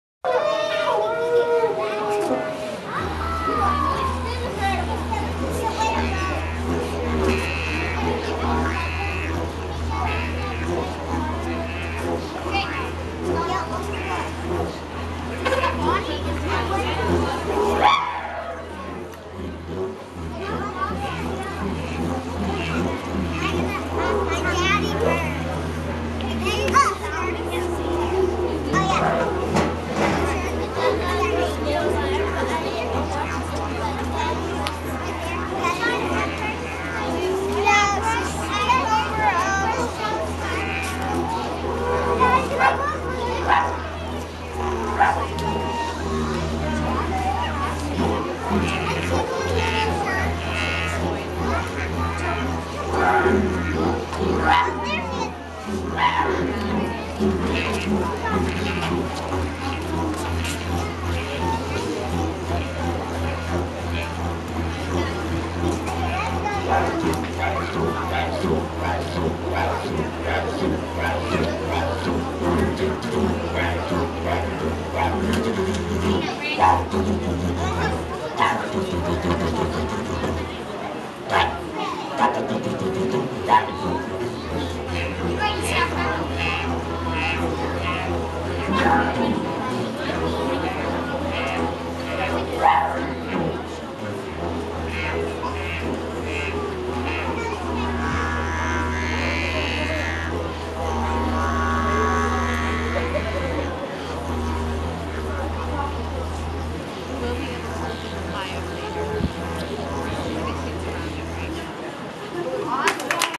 Digerido 2
More playing a Digerido (sp?) at our Australia-themed VBS.